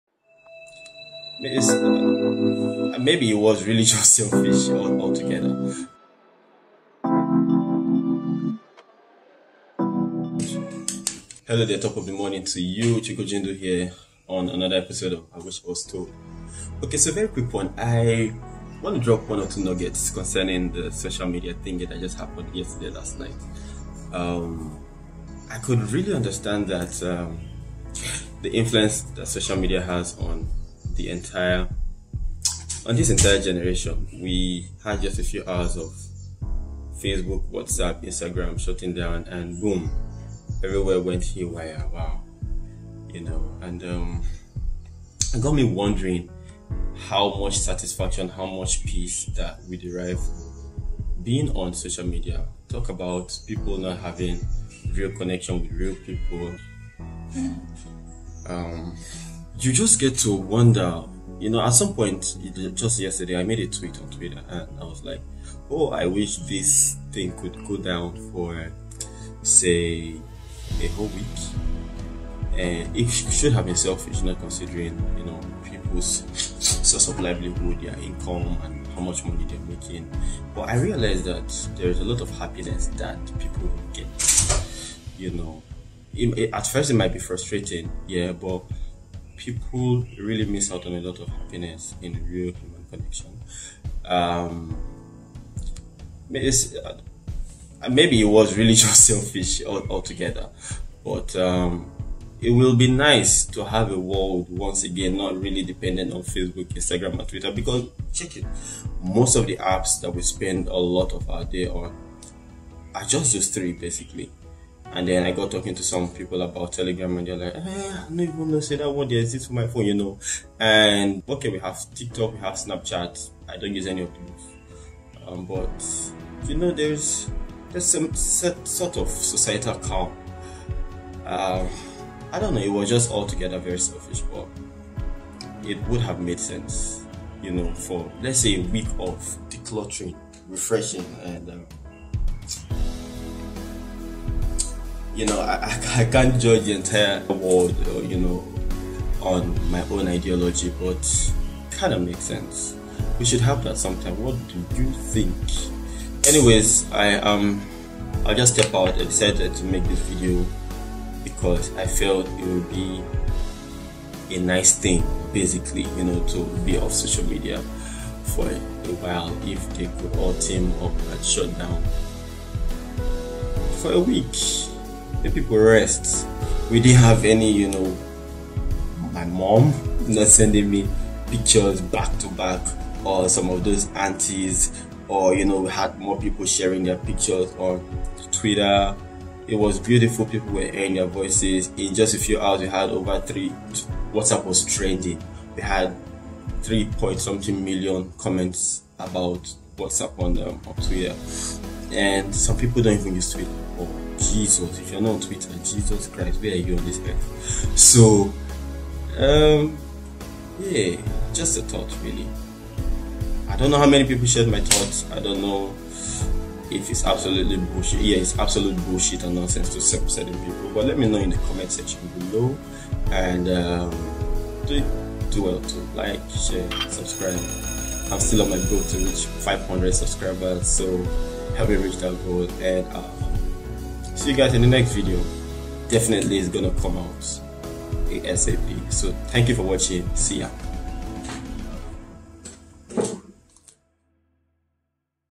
FACEBOOK, INSTAGRAM & WHATSAPP SHUTDOWN | My Selfish Take
Why's these apps so important to us anyway? so much that we "Cannot live without them"? Even my colleague says 'people will die' if these media are shutdown for a week LMAO... I can imagine the depression for same platforms causing oppression.

In this video, I'm thinking out loud and air my thought concerning the 04-10-21 event of the social giant, Facebook experiencing a temporary global shutdown.